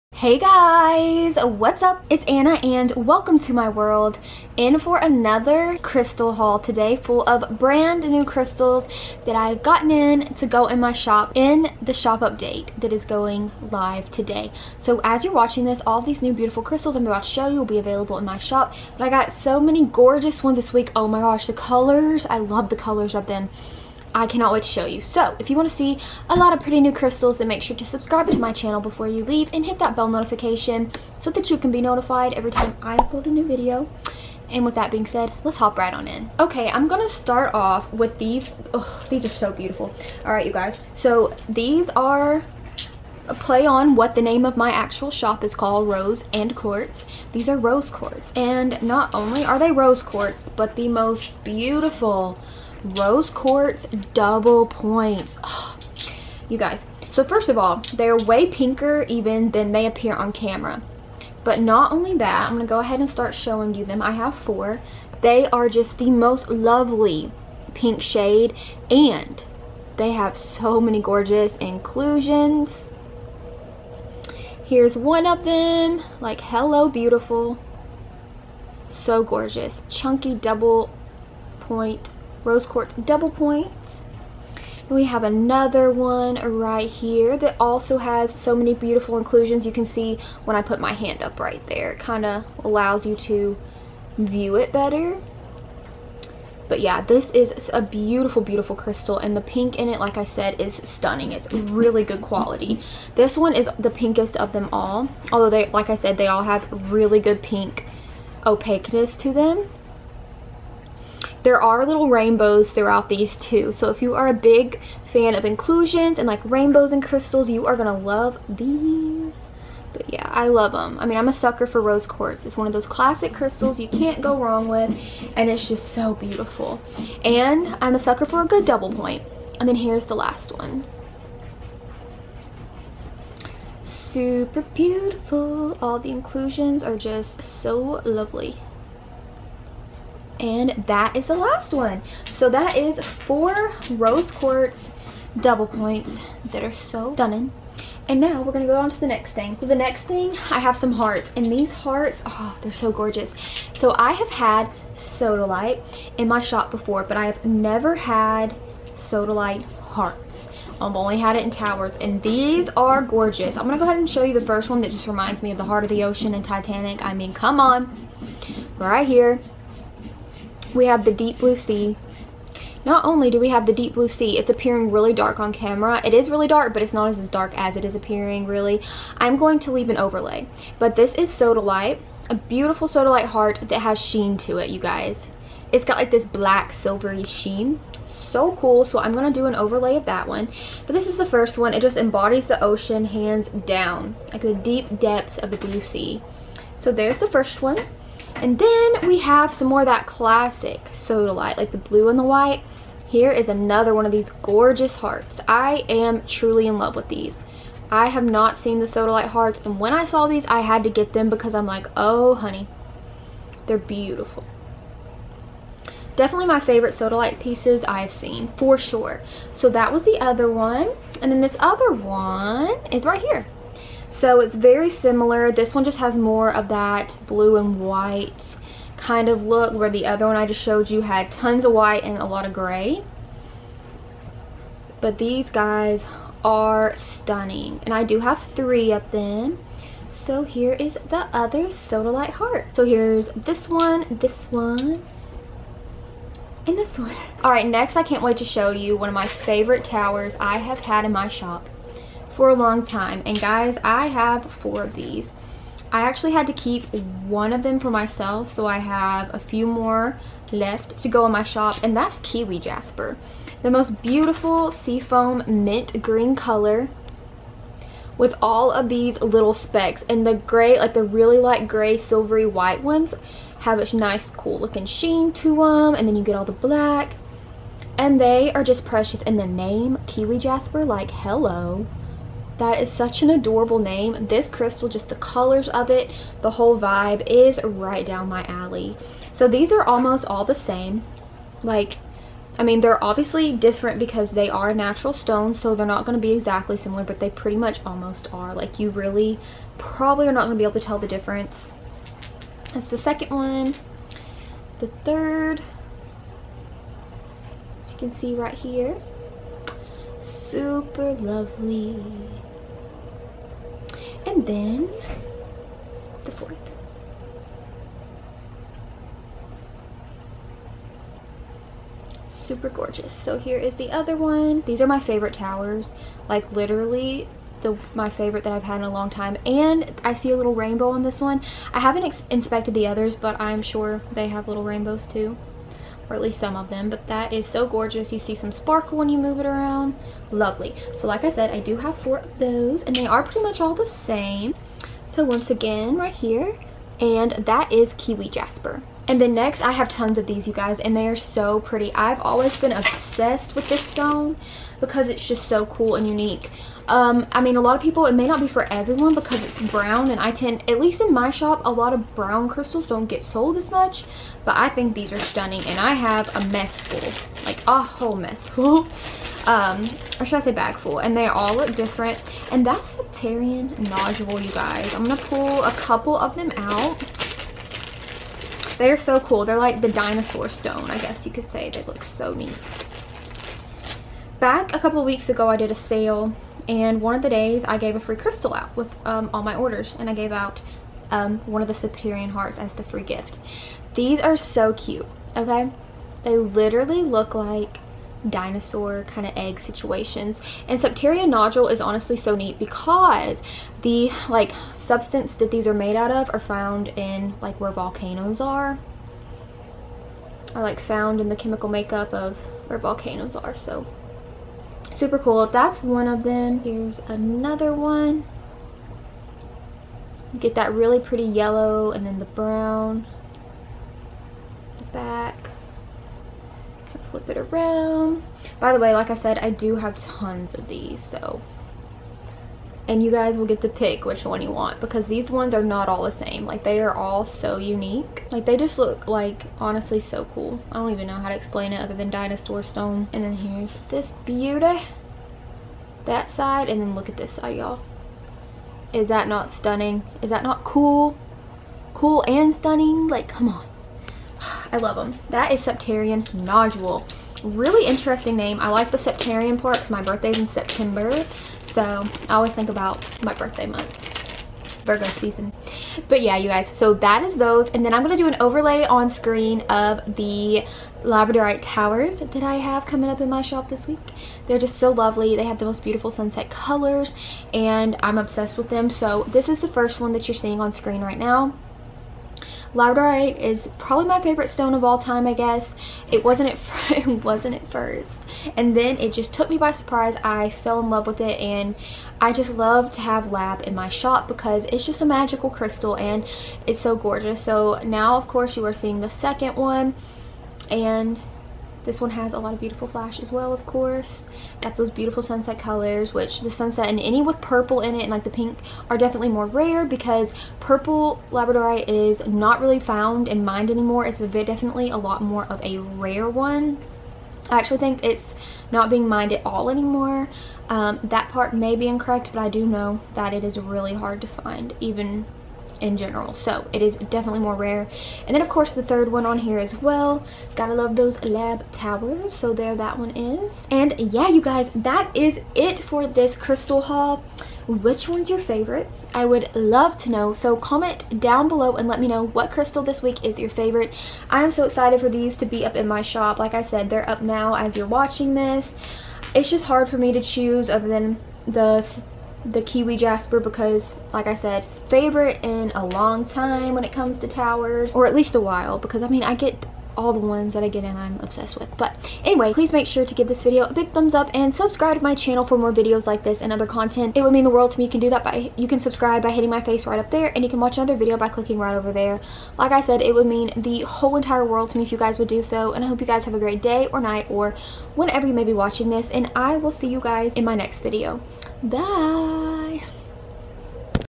CRYSTAL HAUL! TONS OF GORGEOUS CRYSTALS!
Hey guys, for today's video, I have another big crystal haul filled with so many beautiful crystals! my online shop I hope you enjoy it!